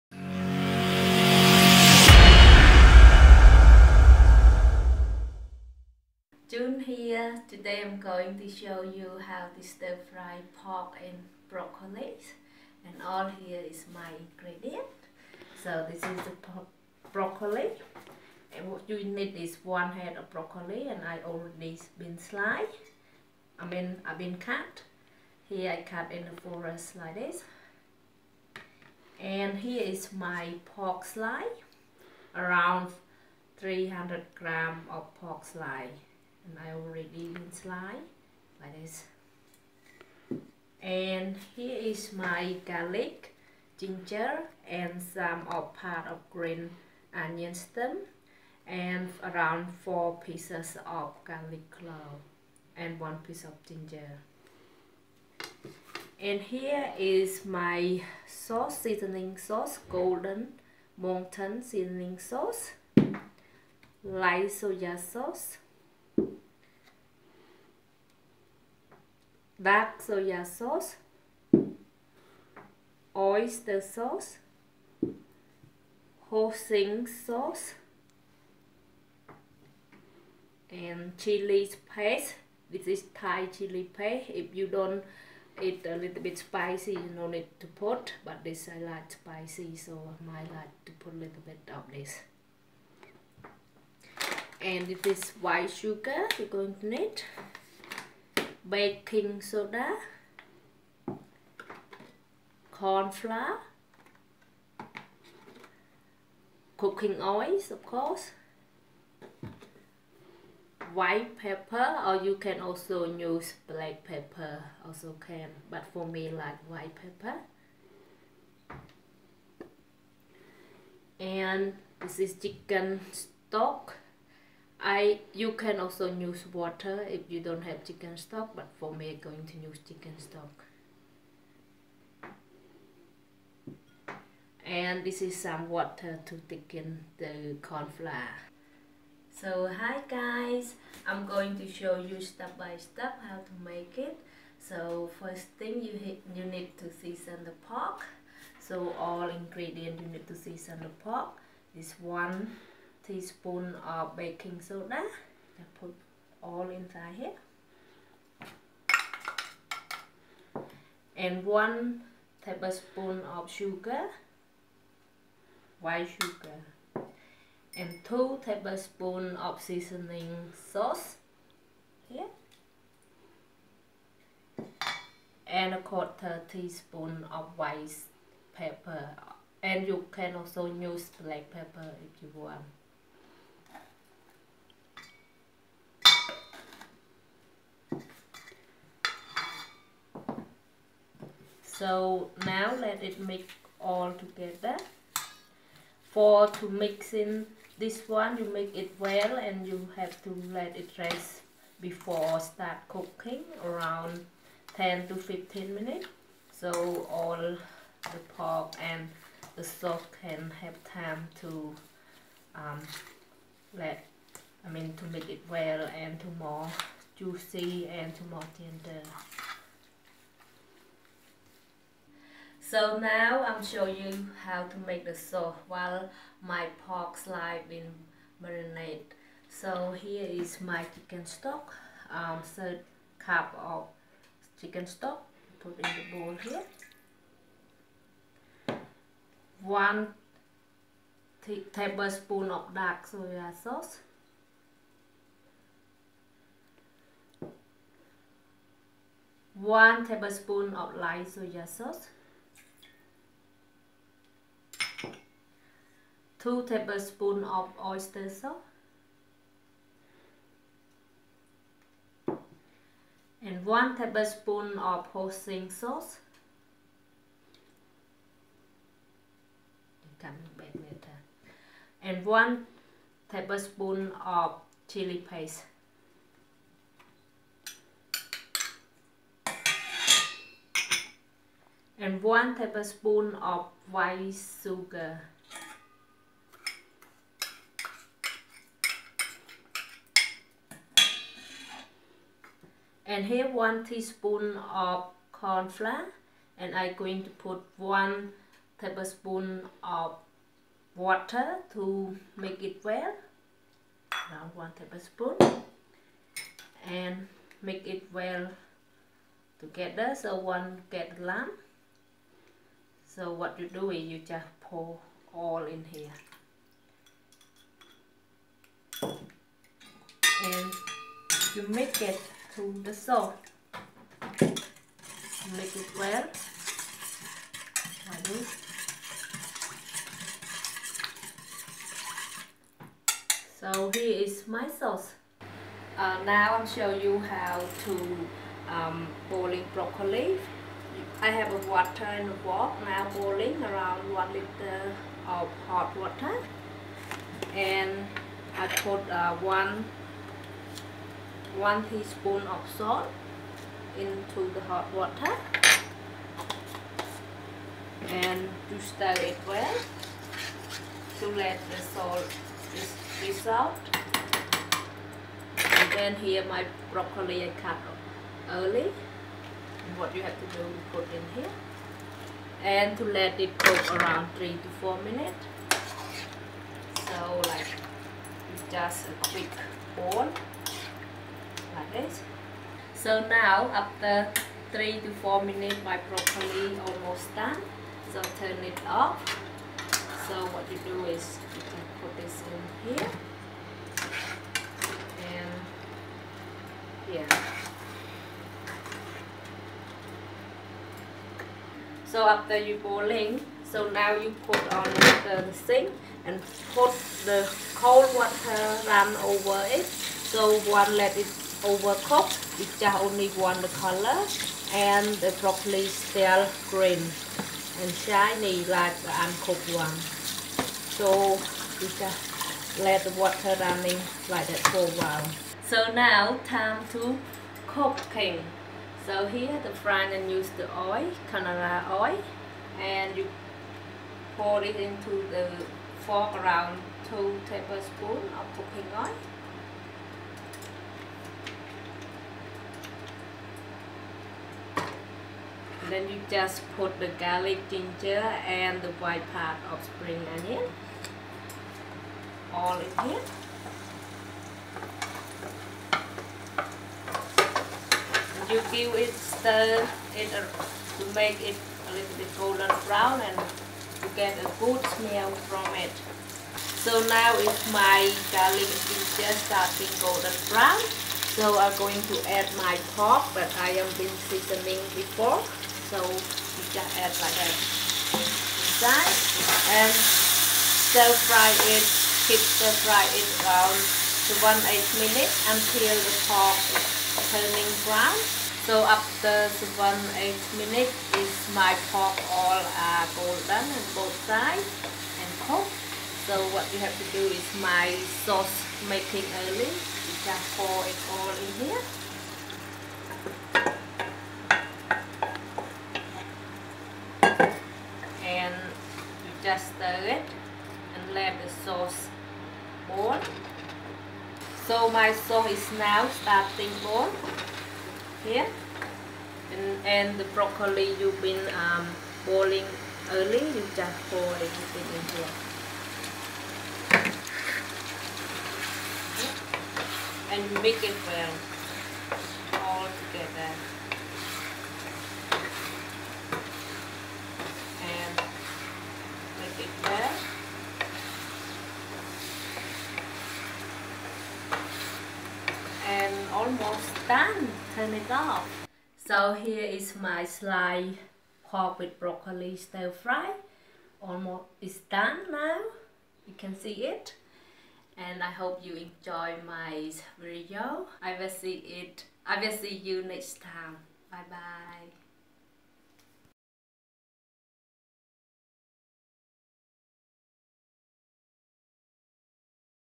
stir fry pork slice with broccoli
In today's Video learn how to make Yummy Stir Fry Pork With Broccoli
🌿🍀🍋Ingredients for seasoning the Pork  🍅🍗🌰
300 grams of pork cut into cubes
2 tablespoons of seasoning sauce (or light soy sauce)
1 tablespoon of white sugar
1 teaspoon of baking soda
¼ teaspoon of white pepper or black pepper

Sauce ingredients
1 cup of chicken stock or water
1 tablespoon of light soy sauce
1 tablespoon of dark soy sauce
2 tablespoons of oyster sauce
1 tablespoon of hoisin sauce
1 tablespoon of chilli paste
1 tablespoon of sugar
1 teaspoon of corn flour
1 tablespoon of water

Ingredients for the stir-fry
1 cup of water
1 large broccoli cut into small pieces
2 tablespoons of cooking oil
4 garlic cloves (chopped)
1 piece of ginger (chopped)
1 stem of green onion (chopped)